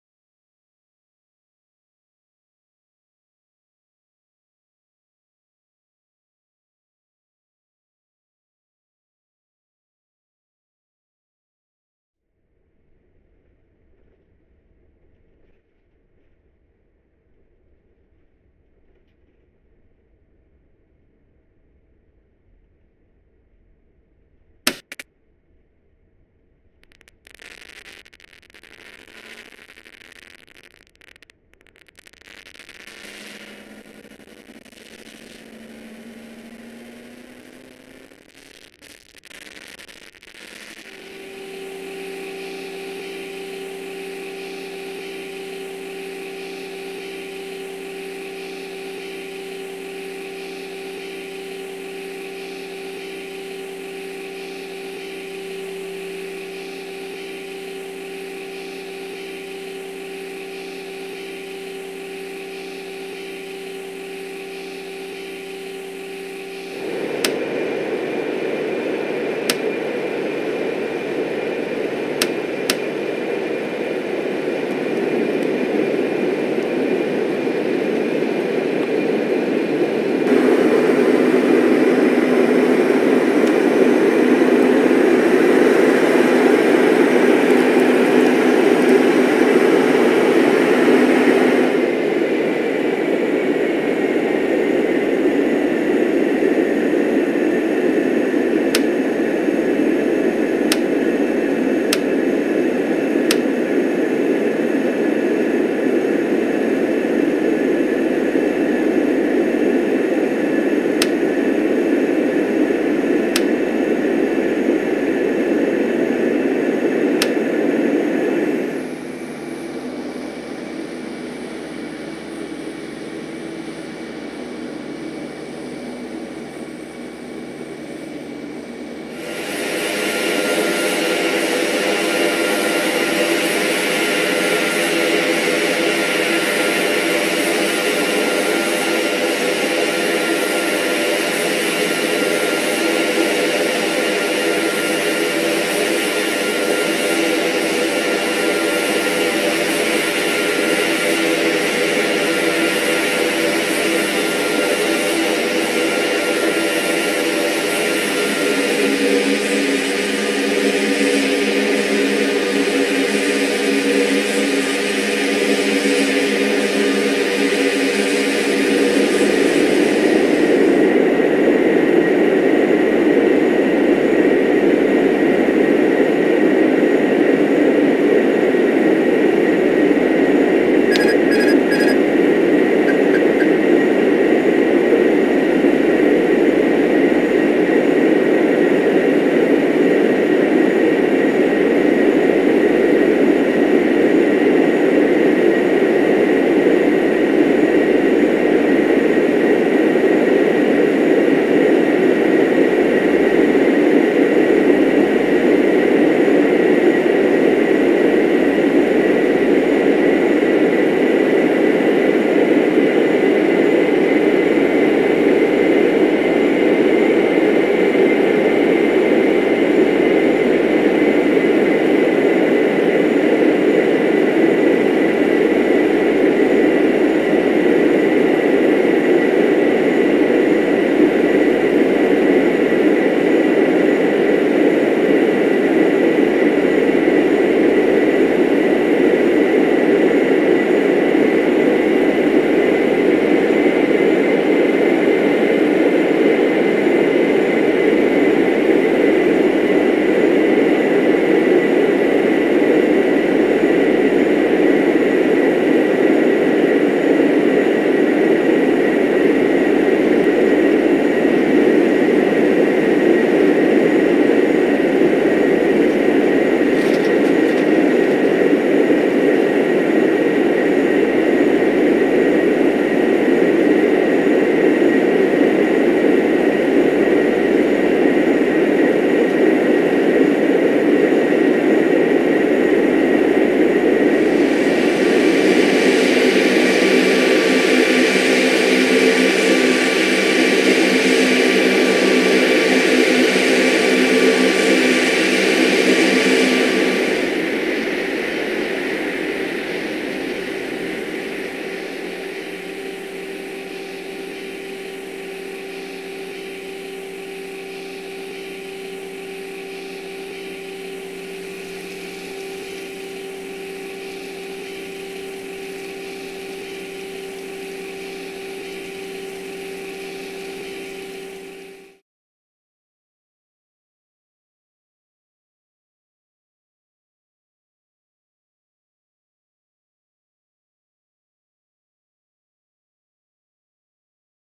Dash 8 Q-400D//SCCI-SCTE//03 10 17
Retornando en este avión desde SCCI hasta SCTE